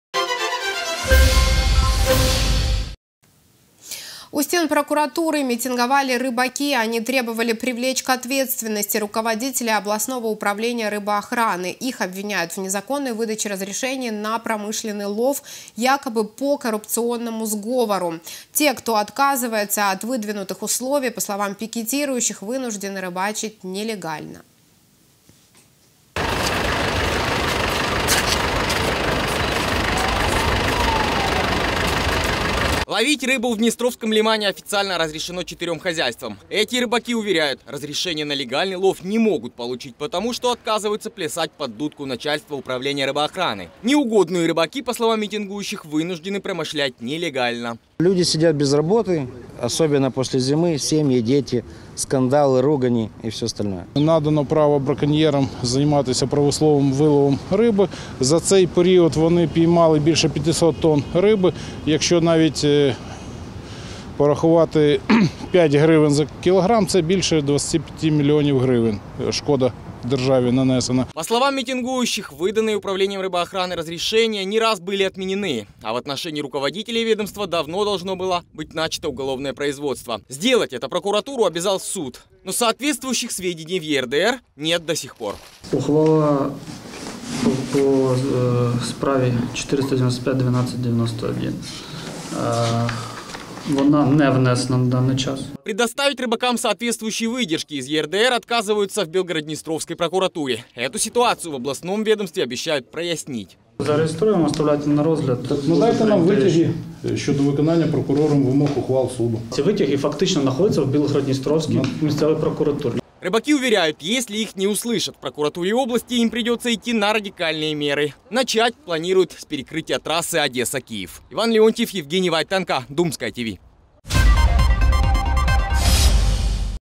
У стен прокуратуры митинговали рыбаки.
Они требовали привлечь к ответственности руководителей областного управления рыбоохраны. Их обвиняют в незаконной выдаче разрешений на промышленный лов, якобы, по коррупционному сговору.